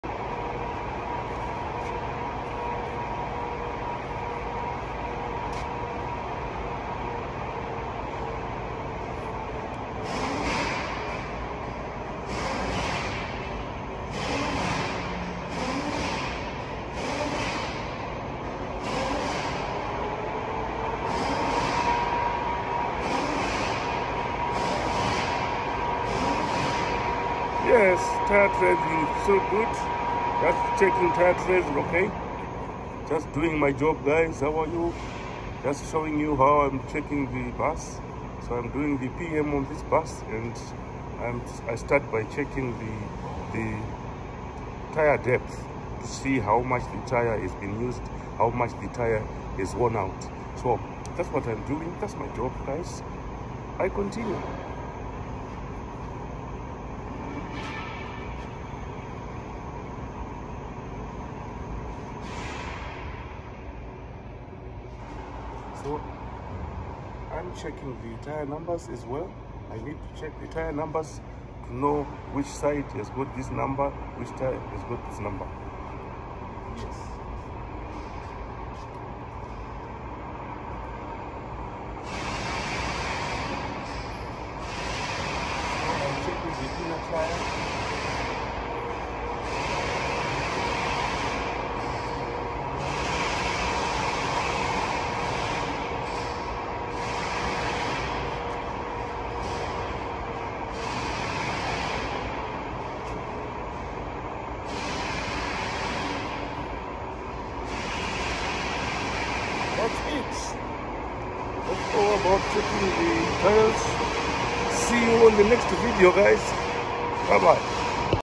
April 28, 2024
Checking tire numbers and thread depth